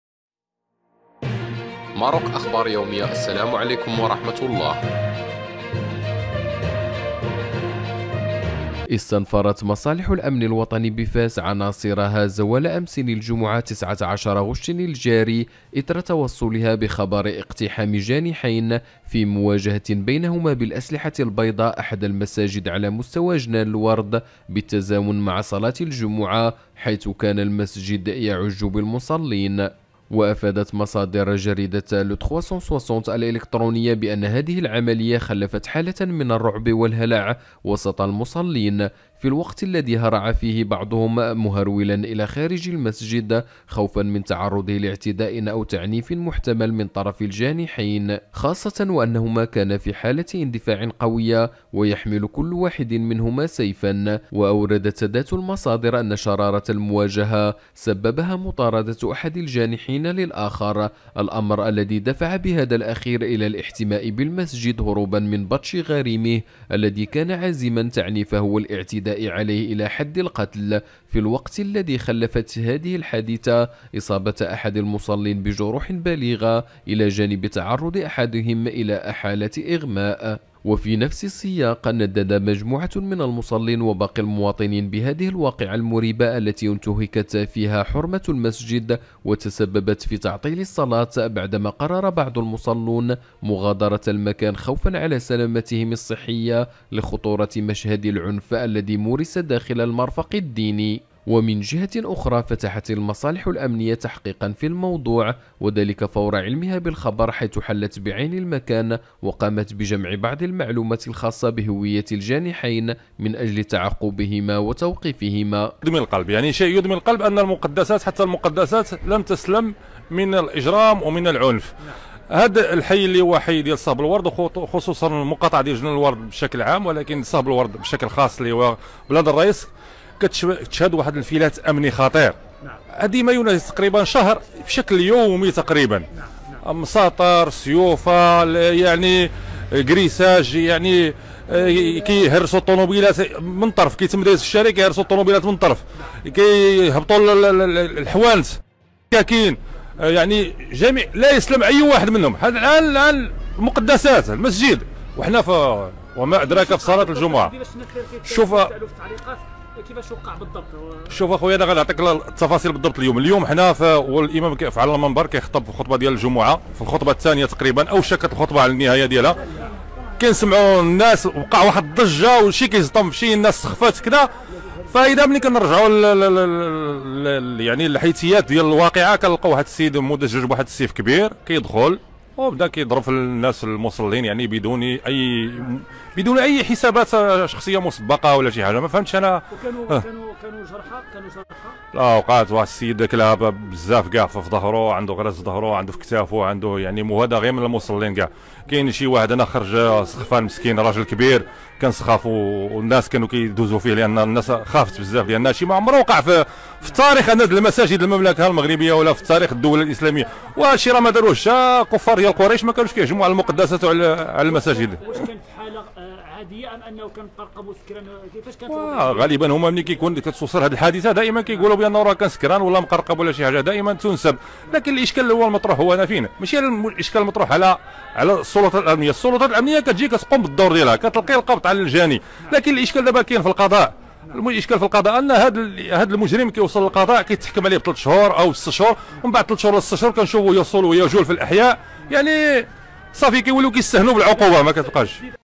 شوفو الروينة لي داروها جوج هازين مسطرات وسط مسجد في فاس
Artiste : __url_artiste__